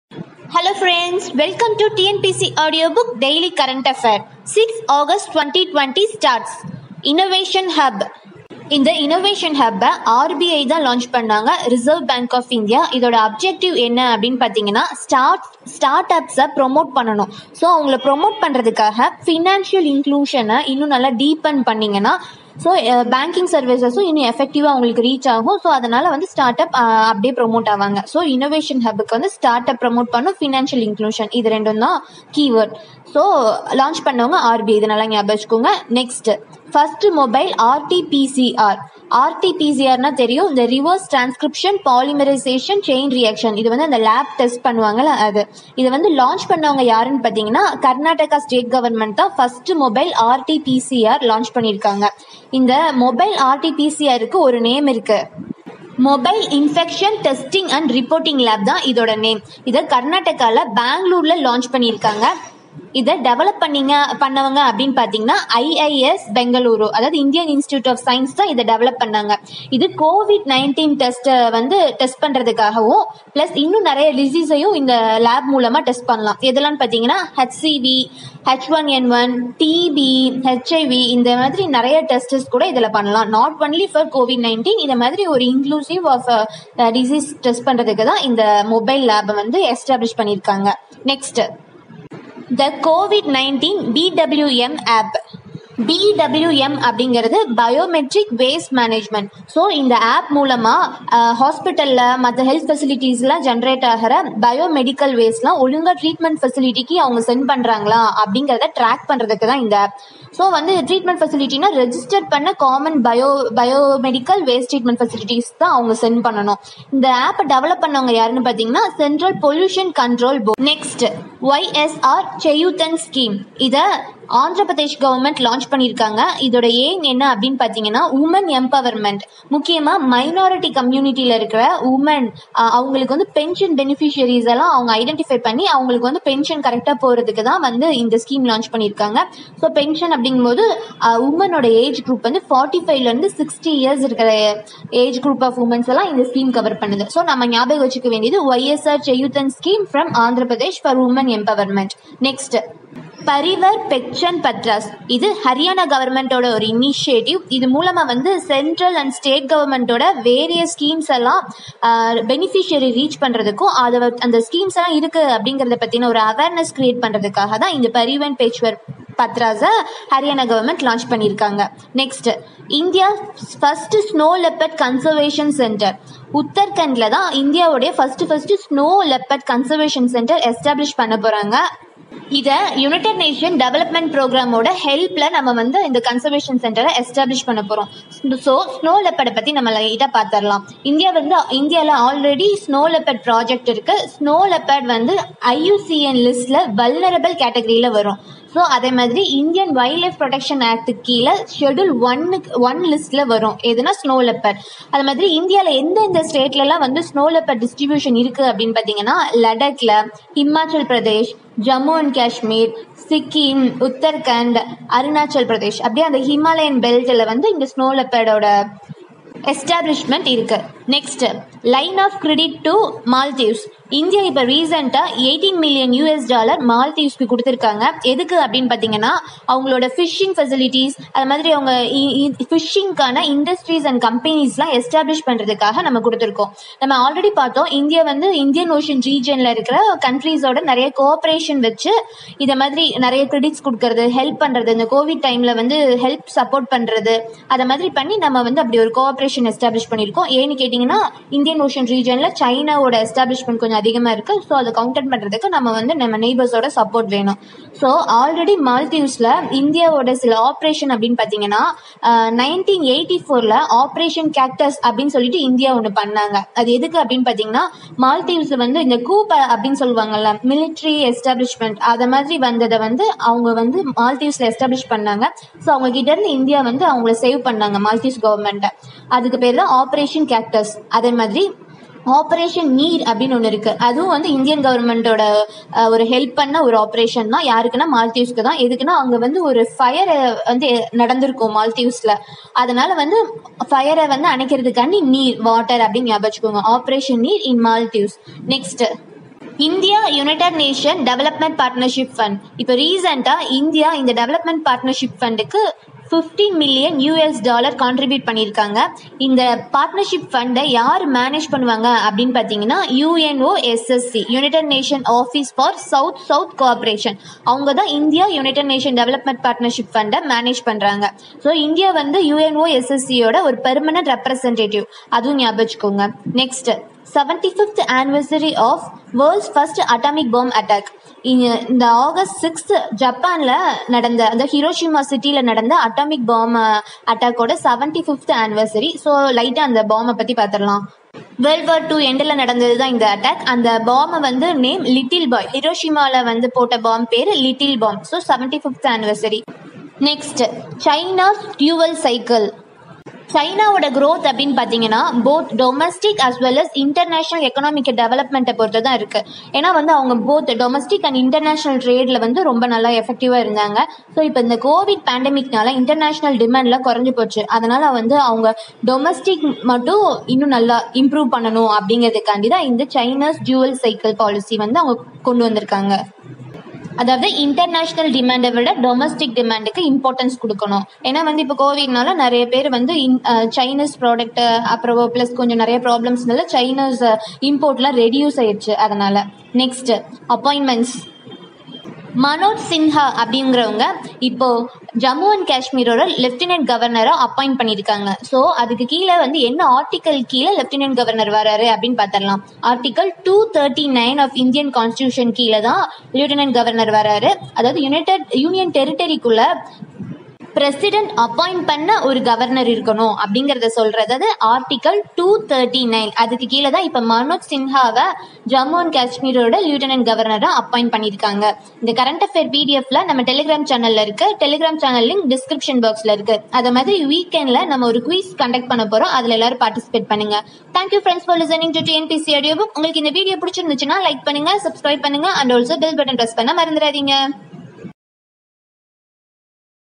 TNPSC-DAILY CURRENT AFFAIRS 06 August 2020
Here is your Daily dose of current affairs of national and international important to brush up your preparation. covers topics of unit 2 in group 1&2  prelims and also covers important topics group 1mains.
The content are taken from variety of reliable sources.

"Let's listen and crack it together"